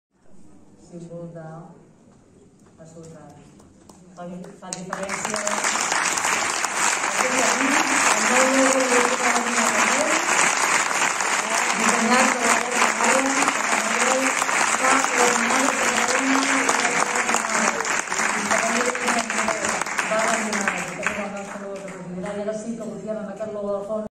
Presentació del nou logo OB L'ATENEU
La presentació del llibre del centenari, celebrada el 26 de març de 2022 al Teatre Margarida Xirgu de Badalona, va estar plena de sorpreses.
Una d'elles va ser donar la benvinguda a un nou logotip que identificarà l'entitat i serà efectiu en acabar la celebració del centenari.